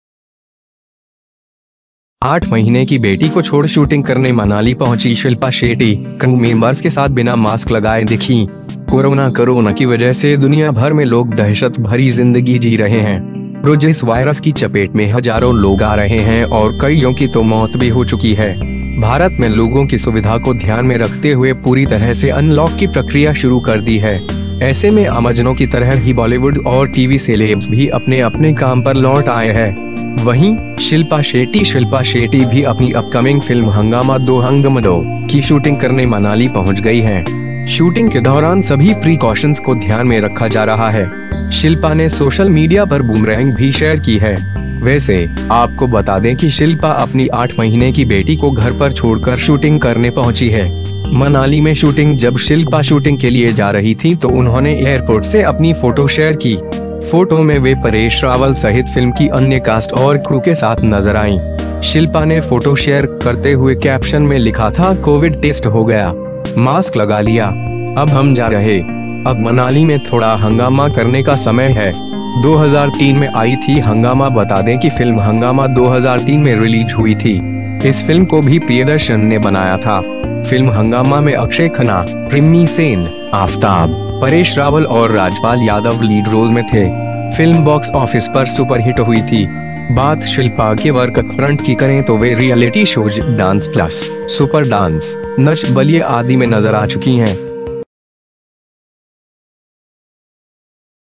8 महीने की बेटी को छोड़ शूटिंग करने मनाली पहुंची शिल्पा शेट्टी, क्रू मेंबर्स के साथ बिना मास्क लगाए
8 महीने की बेटी को छोड़ शूटिंग करने मनाली पहुंची शिल्पा शेट्टी, क्रू मेंबर्स के साथ बिना मास्क लगाए दिखीं

कोरोना (corona) की वजह से दुनियाभर में लोग दहशत भरी जिंदगी जी रहे हैं। रोज इस वायरस की चपेट में हजारों लोग आ रहे हैं और कईयों की तो मौत भी हो चुकी है। भारत में लोगों की सुविधा को ध्यान में रखते हुए पूरी तरह से अनलॉक की प्रक्रिया शुरू कर दी है। ऐसे में आमजनों की तरह ही बॉलीवुड और टीवी सेलेब्स भी अपने-अपने काम पर लौट आए है। वहीं, शिल्पा शेट्टी (shilpa shetty) भी अपनी अपकमिंग फिल्म हंगामा 2 (hungama 2) की शूटिंग करने मनाली पहुंच गई हैं। शूटिंग के दौरान सभी प्रीकॉशन्स को ध्यान में रखा जा रहा है। शिल्पा ने सोशल मीडिया पर बूमरैंग भी शेयर की है। वैसे, आपको बता दें कि शिल्पा अपनी 8 महीने की बेटी को घर पर छोड़कर शूटिंग करने पहुंची है।

मनाली में शूटिंग
जब शिल्पा शूटिंग के लिए जा रही थीं तो उन्होंने एयरपोर्ट से अपनी फोटो शेयर की। फोटो में वे परेश रावल सहित फिल्म की अन्य कास्ट और क्रू के साथ नजर आईं। शिल्पा ने फोटो शेयर करते हुए कैप्शन में लिखा था- कोविड टेस्ट हो गया। मास्क लगा लिया। अब हम जा रहे। अब मनाली में थोड़ा हंगामा करने का समय है।

2003 में आई थी हंगामा
बता दें कि फिल्म हंगामा 2003 में रिलीज हुई थी। इस फिल्म को भी प्रियदर्शन ने बनाया था। फिल्म हंगामा में अक्षय खन्ना, रिमी सेन, आफताब, परेश रावल और राजपाल यादव लीड रोल में थे। फिल्म बॉक्स ऑफिस पर सुपर हिट हुई थी। बात शिल्पा के वर्कफ्रंट की करें तो वे रियलिटी शोज डांस प्‍लस, सुपर डांस, नच बलिए आदि में नजर आ चुकी हैं।